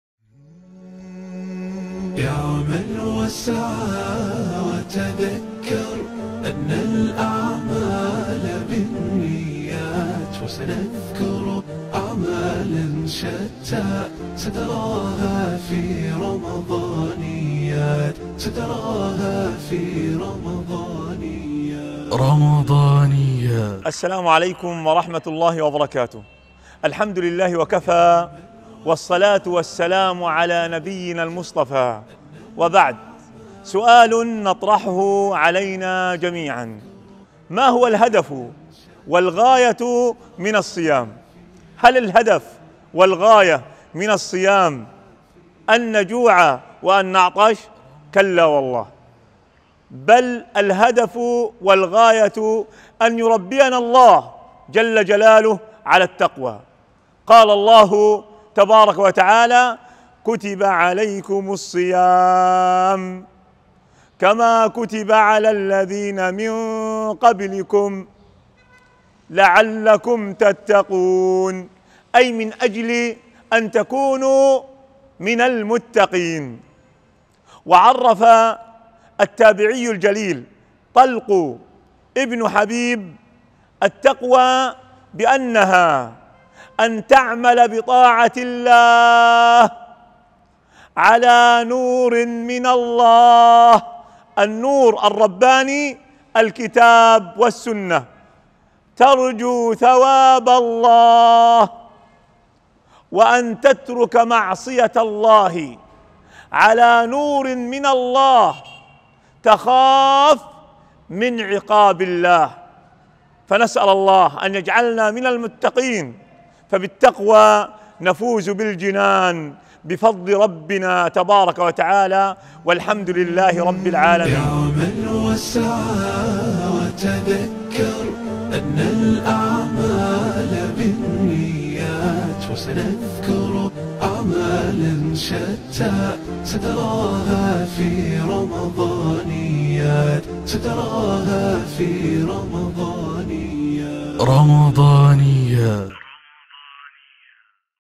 ما هو الهدف والغاية من الصيام ؟! للشيخ إسلام أبو سعيّد
أهلا بك في قناتنا عبر يوتيوب
نرجوا منك دعمنا عبر متابعتك ومشاركتك
يوتيوب